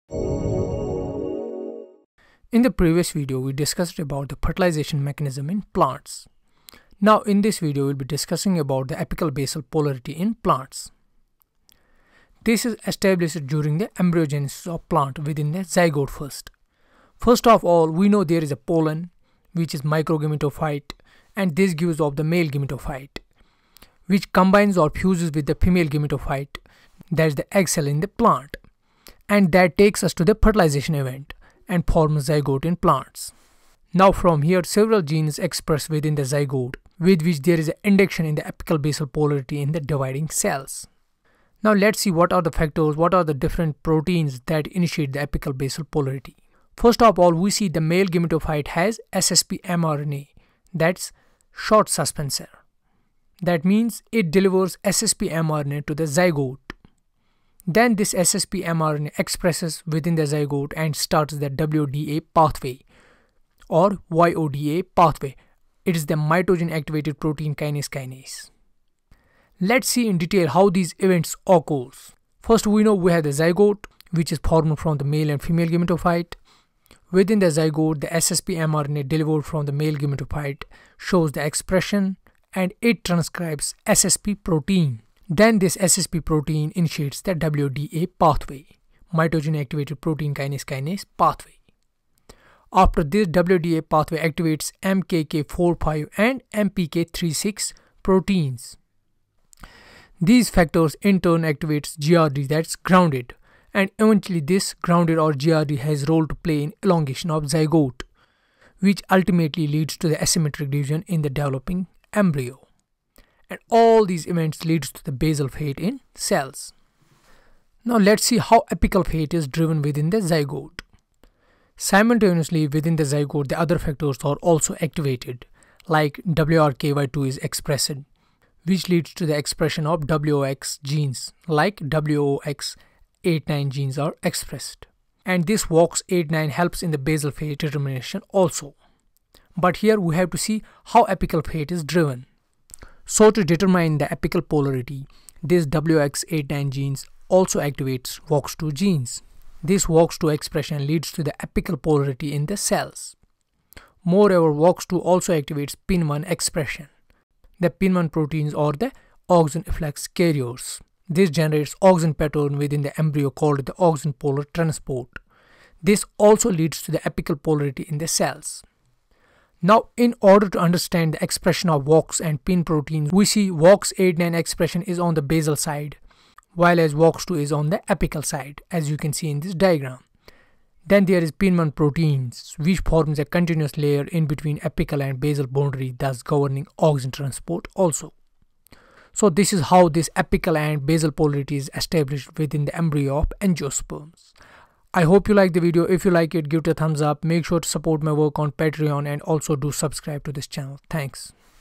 Apical Basal Polarity | YDA-SSP Pathway in Plants
In this Video we have discussed the apical basal polarity in plants.
We know after the fertilization in plants the zygote acquires Apical and basal polarity. The Apical polarity and basal polarity is achieved by the different proteins and genes within the zygote.